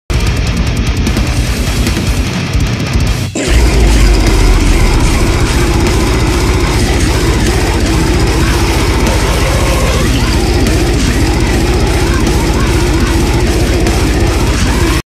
CJ McCreery Evolution 1: Signs of the Swarm (2014-2018)
Yeyo everybody, this is my 149th video, Part 1 of CJ McCreery's evolution, starting with his first band, Signs of the Swarm, which he formed in 2014, made 2 albums with, and left in 2018 for Lorna Shore.